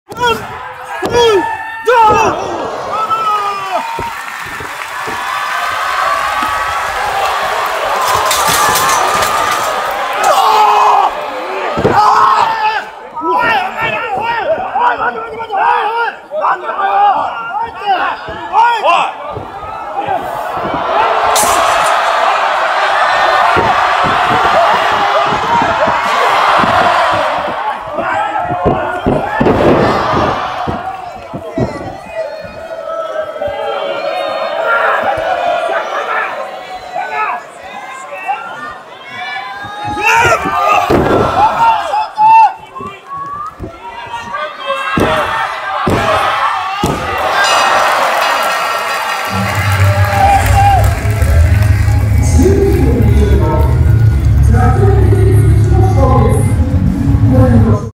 【新日本プロレス】海野翔太 vs ジャック・ペリー、まさかの乱入者…！
2024.3.6 東京・大田区総合体育館
旗揚げ記念日